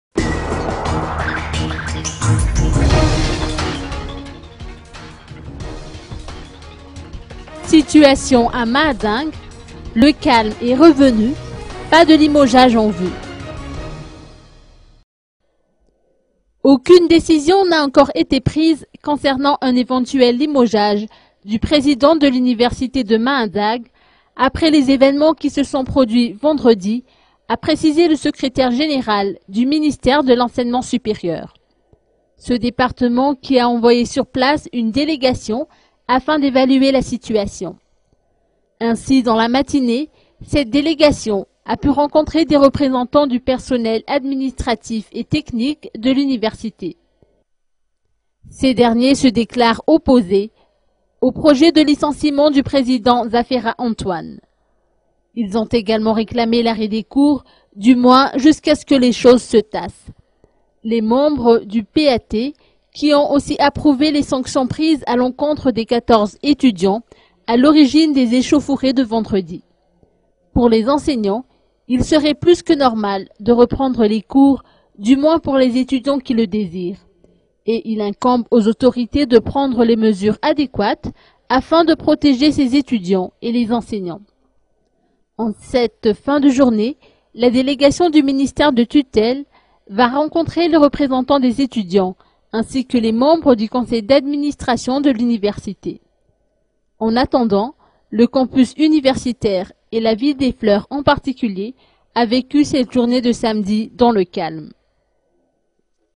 01 SUIVI MAJUNGA
Aucune décision n' a encore été prise concernant un éventuel limogeage du président de l'Université de Mahajanga après les événements qui se sont produits vendredi, a précisé le secrétaire général du ministère de l'Enseignement supérieur.